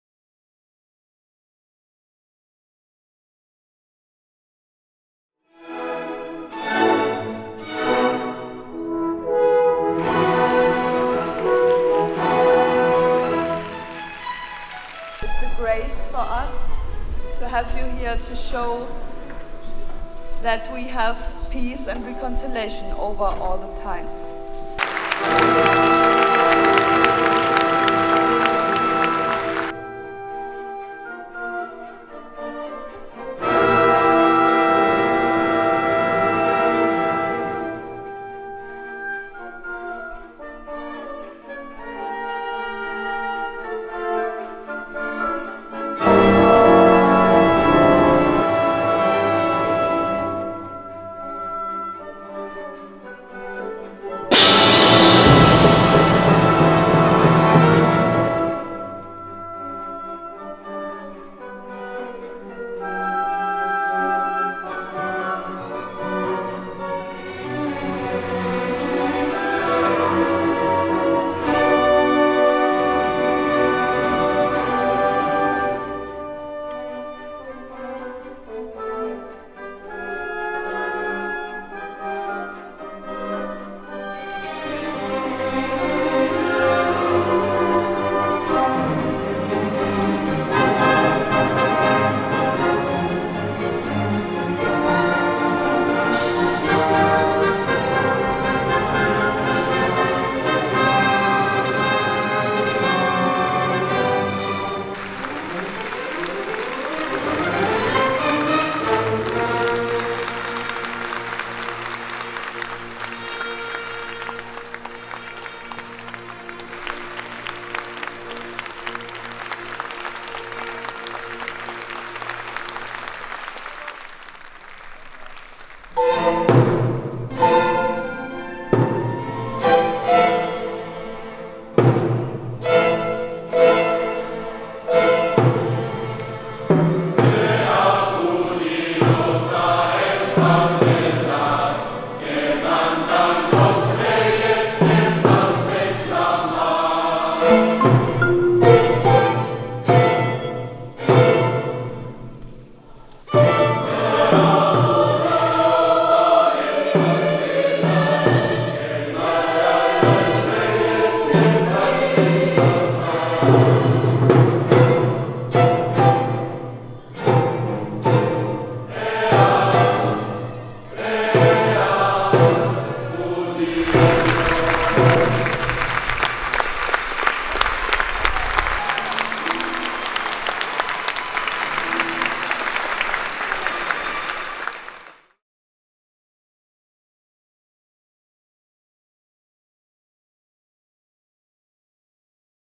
Thelma Yellin Classical Music Department at the Holocaust Memorial Service
27.1.20
מגמת המוסיקה הקלאסית של תלמה ילין השתתפה בטקס הזכרון המרכזי של העיר ברלין לציון יום השואה הבין לאומי בכנסיית הזכרון שבעיר.
תזמורת ומקהלת בית הספר בניצוחם של גיא פדר וישי שטקלר ביצעו יצירות מאת
מהלר, בטהובן, סרוסי ובראון.
סיור הקונצרטים לגרמניה שהתקיים באותו החורף היה שיתוף פעולה יוצא דופן  בין הנהגת ההורים המרכזית והורי תלמידי המגמה לבין אישי ממשל וגופים שבחרו לתמוך ב103 הצעירים שיצאו לציין את זכרון השואה דרך המוסיקה ורוח האדם.

מנהל הסיור - דני דונר
על הוידאו - הדר ברדה
ראש המגמה למוסיקה קלאסית - משה אהרונוב סרוק